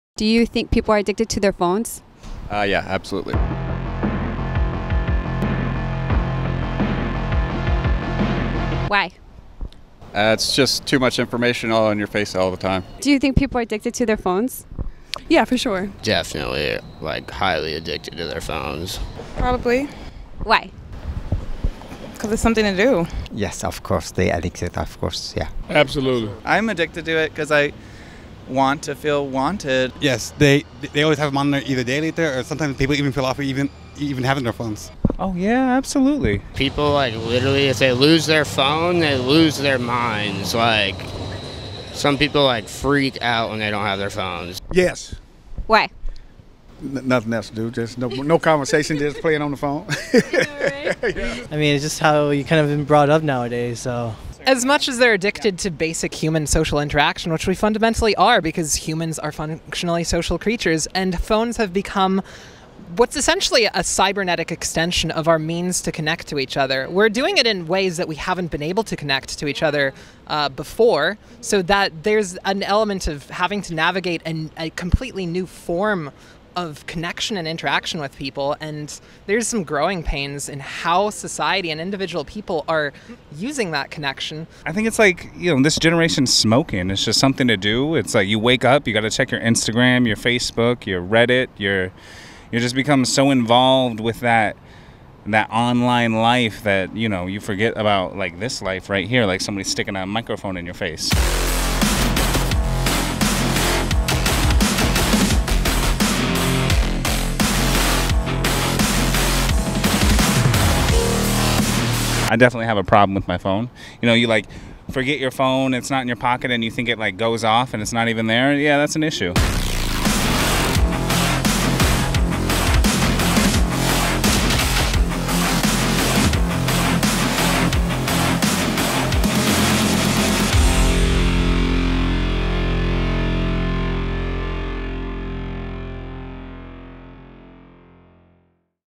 Are People Addicted to Their Phones?
We asked 12 people one simple question.  Are people addicted to their phones?

Here's what they have to say.

What about you?  Tell us what you think.